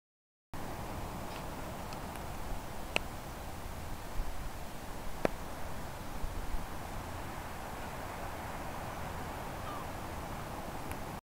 MPEG0101 Schiff bei Beira, Nordküste Sao Jorge
Aufgenommen am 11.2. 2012 um 10:37 an der Südküste von Sao Jorge, Azoren, Zentralgruppe
gespeichert in Sao Jorge 3